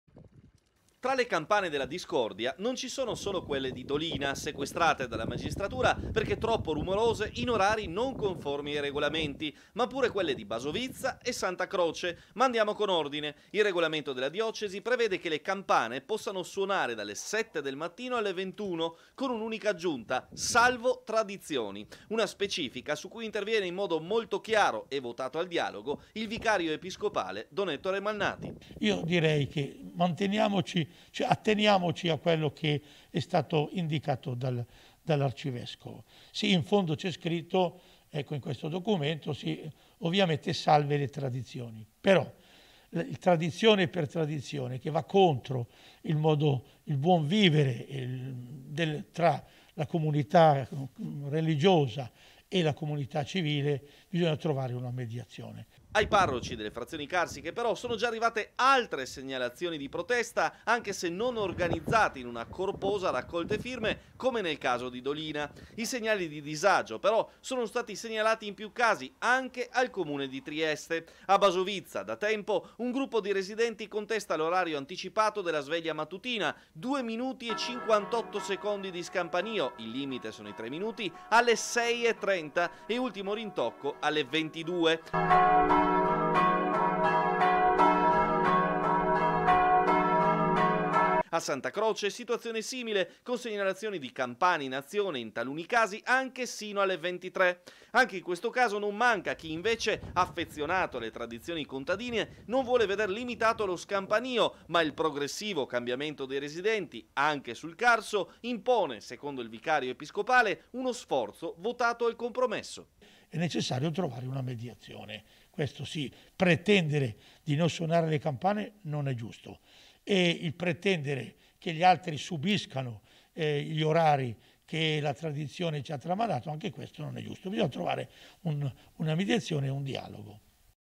LE CAMPANE DELLA DISCORDIA: PROTESTE ANCHE A BASOVIZZA E SANTA CROCE | 14/01/2022
TG sera 14/01/2022 - TRIESTE - Non c'è solo il caso di Dolina, tra quelli di campane che suonano fuori dall'orario previsto dalla Diocesi tra le frazioni carsiche triestine. Anche a Basovizza e Santa Croce segnalazioni e proteste, nel recente passato, non sono mancate pur non arrivando ad un sequestro come nel caso di questi giorni. Il vicario episcopale, monsignor Ettore Malnati, ai nostri microfoni ha sottolineato la necessità di trovare un dialogo tra parroci e residenti.  - Intervistati DON ETTORE MALNATI (VICARIO EPISCOPALE) (Servizio di Marco Stabile) - Segui Telequattro anche sul digitale terrestre! Visita il sito